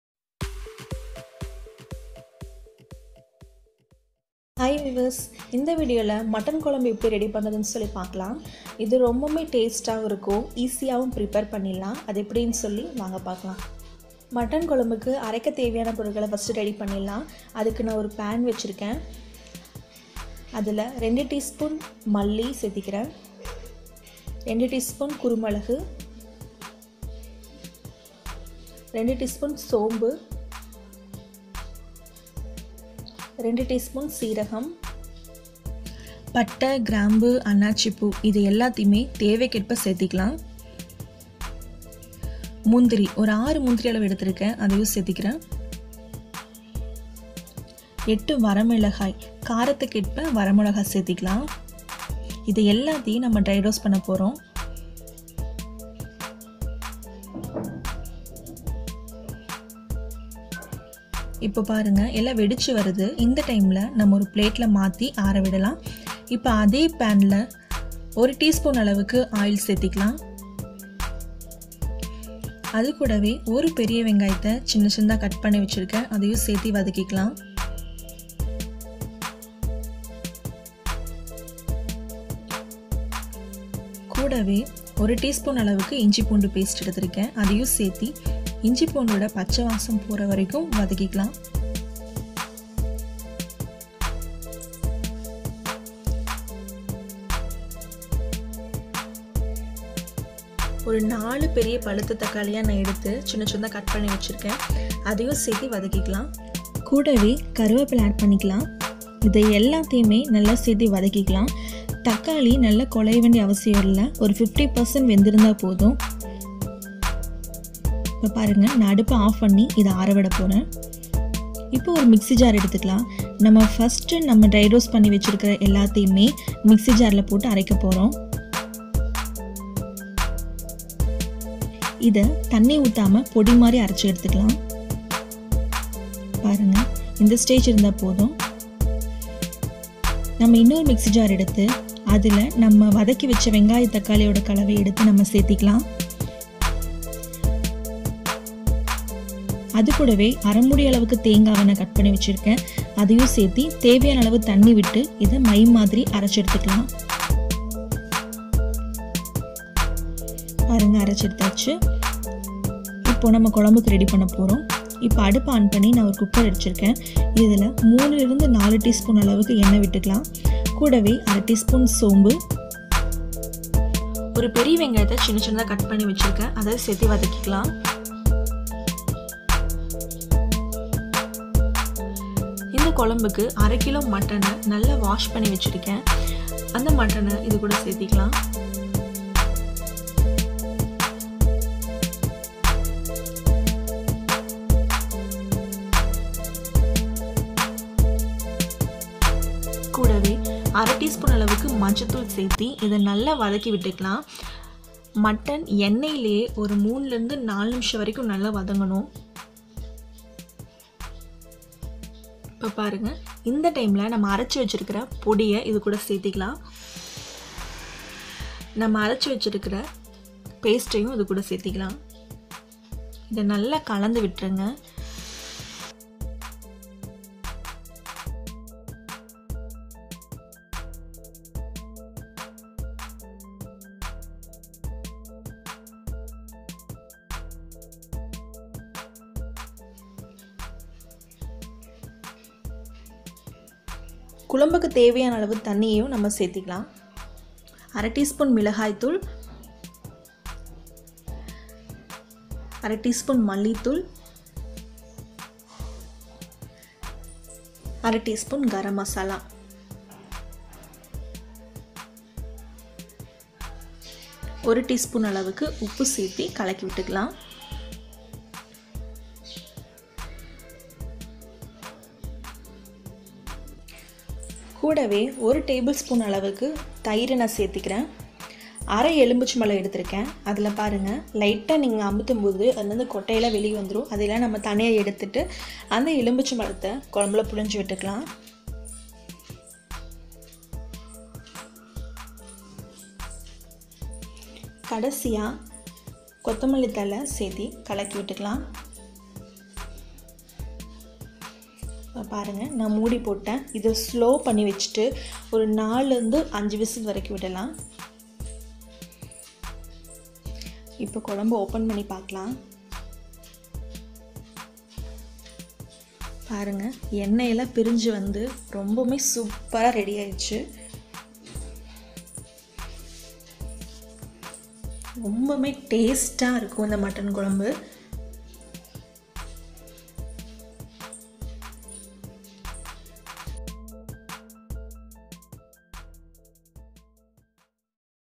கிராமத்து ஸ்டைல் மட்டன் குழம்பு / village style mutton kulambu
Mutton kulambu is very tasty and different